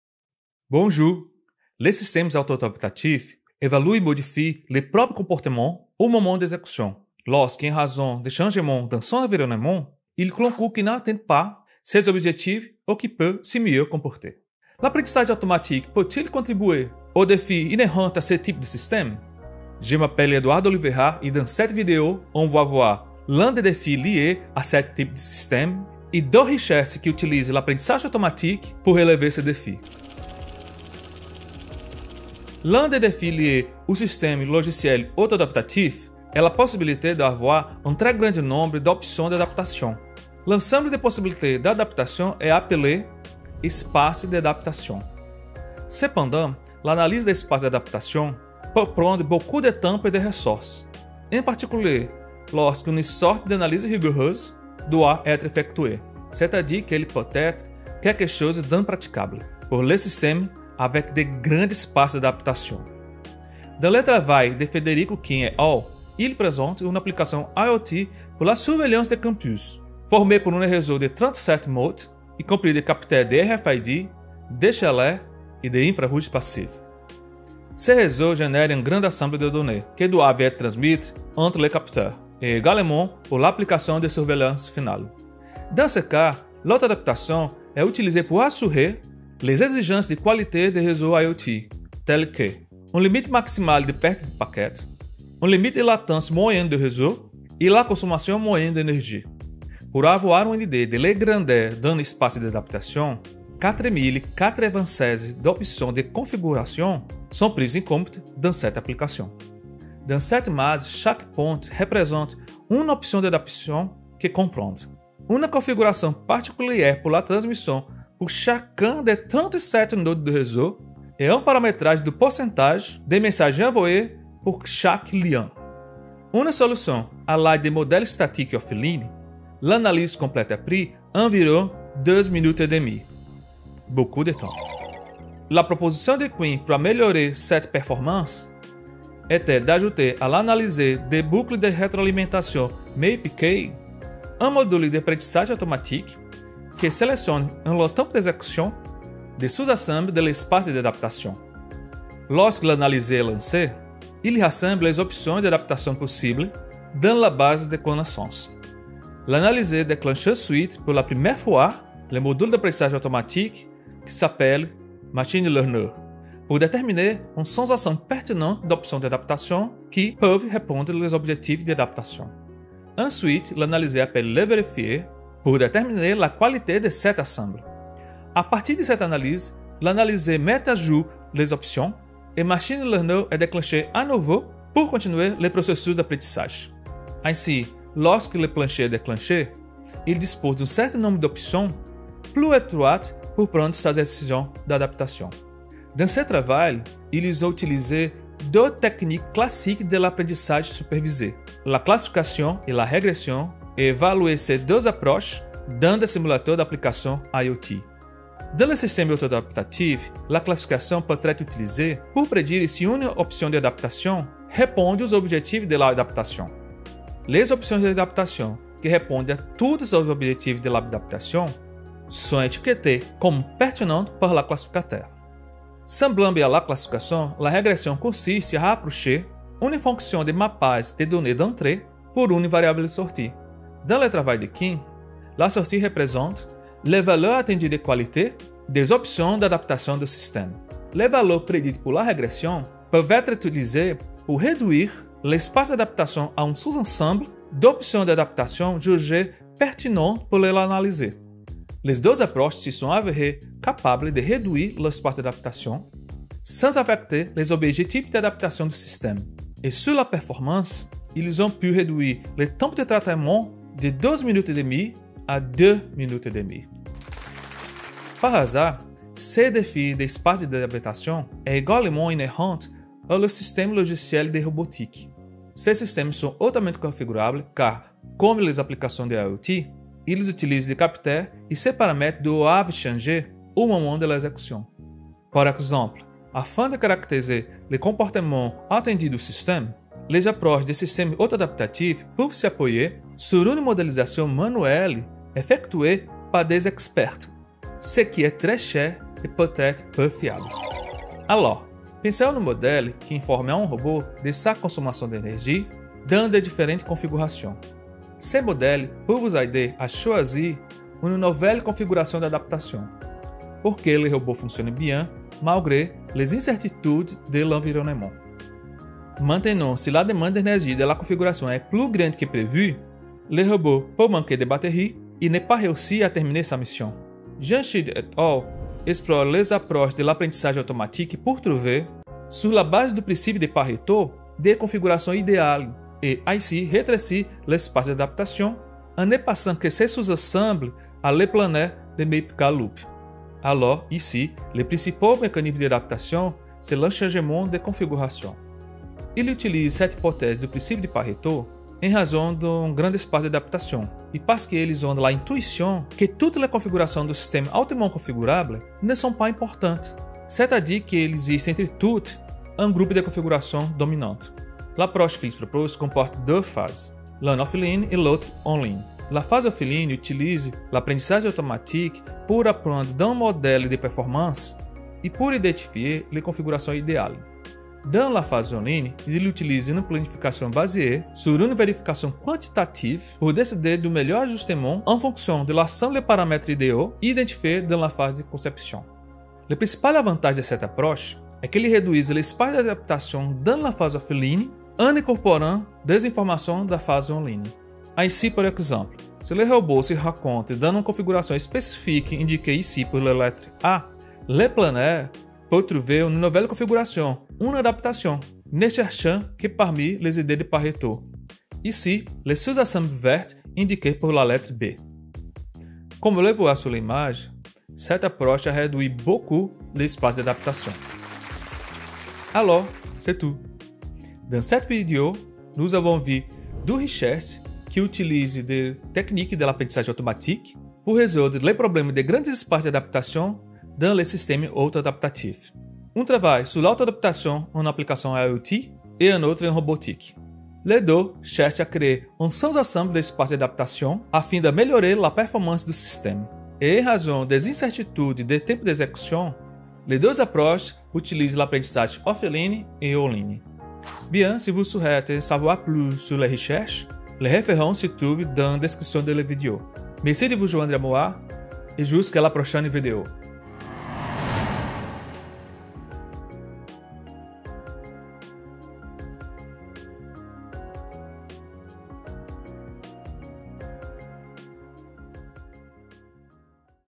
Apprentissage Automatique et Systèmes Auto-Adaptatifs
Les systèmes logiciels auto-adaptatifs évaluent et modifient leur propre comportement, au moment de l'exécution, lorsque en raison des changements dans son environnement, il conclut qu'il n'atteint pas ses objectifs ou qu'il peut mieux se comporter.

L'apprentissage automatique peut-il contribuer aux défis inhérents à ce type de système?

# Références

[1] De Lemos, Rogério, et al. "Software engineering for self-adaptive systems: A second research roadmap." Software Engineering for Self-Adaptive Systems II. Springer, Berlin, Heidelberg, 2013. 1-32.

[2] Laddaga R. Active software. InInternational Workshop on Self-Adaptive Software 2000 Apr 17 (pp. 11-26).
Springer, Berlin, Heidelberg.

[3] Federico Quin, Thomas Bamelis, Singh Buttar Sarpreet, and Sam Michiels. 2019. Efficient analysis of large adaptation spaces in self-adaptive systems using ma-chine learning. In 2019 IEEE/ACM 14th International Symposium on SoftwareEngineering for Adaptive and Self-Managing Systems (SEAMS). IEEE, 1–12.

[4] Pooyan Jamshidi, Javier Cámara, Bradley Schmerl, Christian Käestner, and DavidGarlan. 2019. Machine learning meets quantitative planning: Enabling self-adaptation in autonomous robots. In2019 IEEE/ACM 14th International Symposiumon Software Engineering for Adaptive and Self-Managing Systems (SEAMS). IEEE, 39–50.